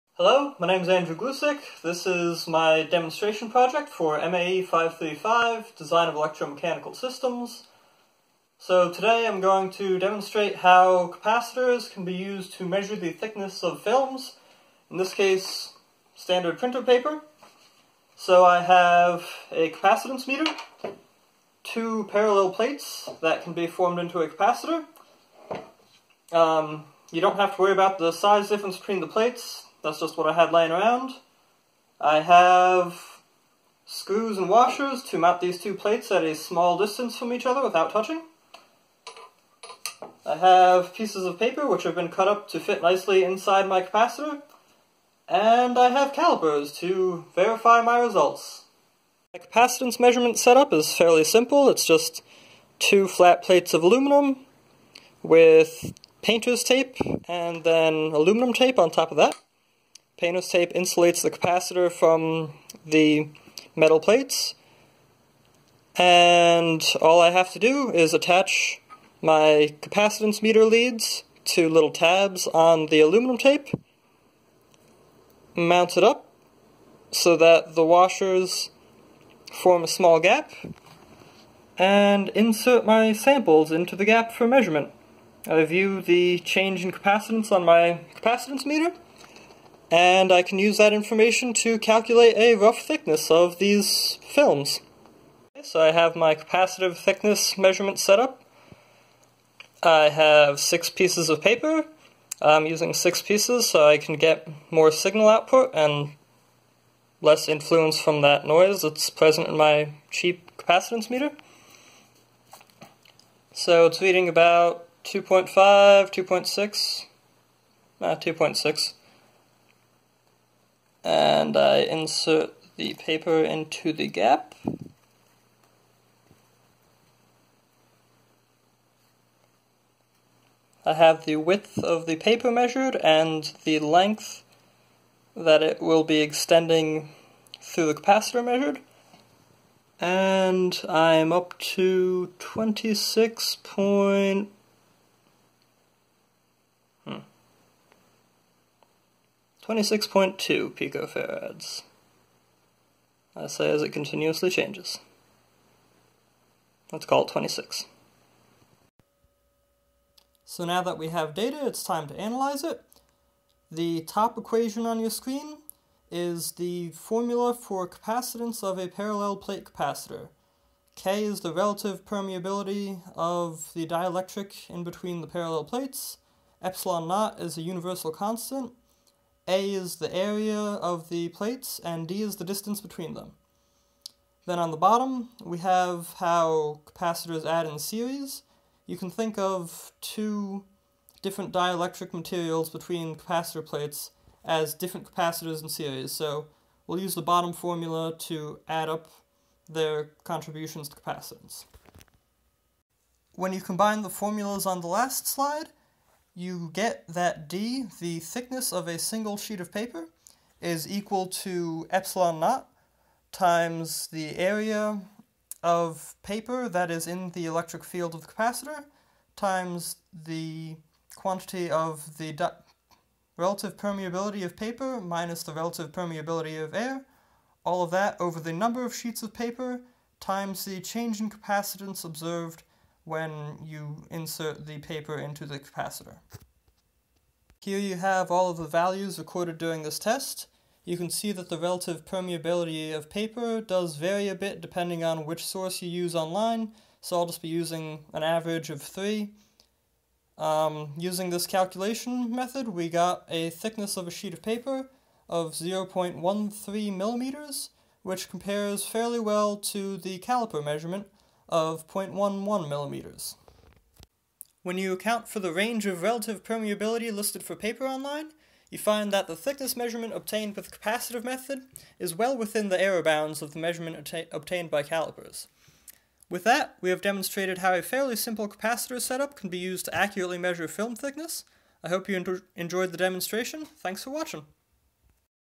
MAE 535 Demonstration Project Dielectric Film Thickness Measurement with Capacitors
This is a short demonstration of using a parallel-plate capacitor to measure the thickness of printer paper.